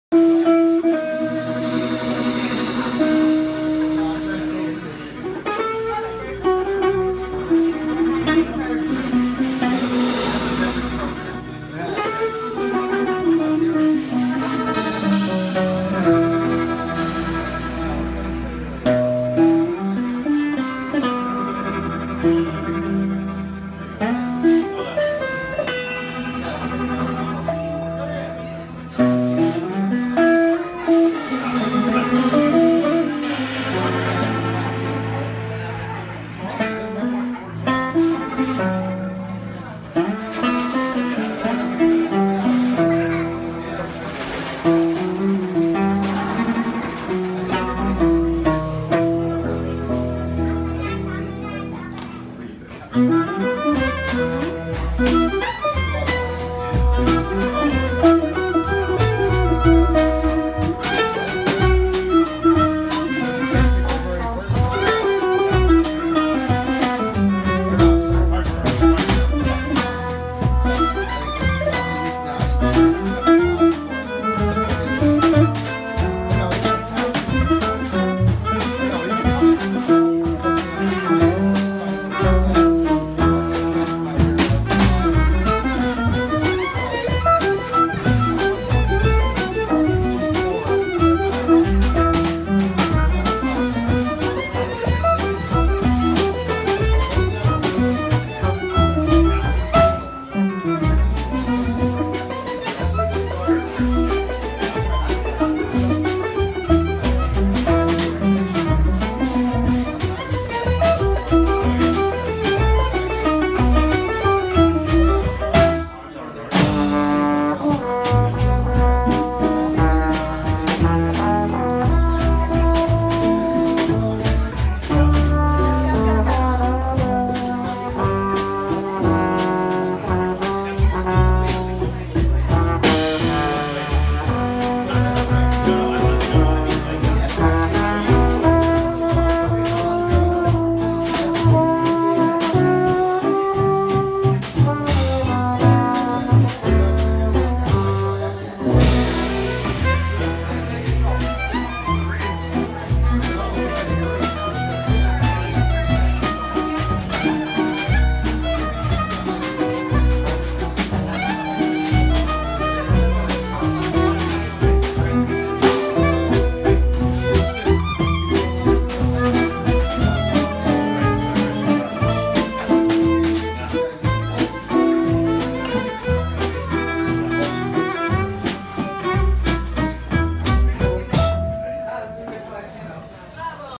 The Lucky 5 - April 30, 2017
The Lucky 5 play Hot Swing Gypsy jazz at the Prairie Whale . . . most Sunday evenings.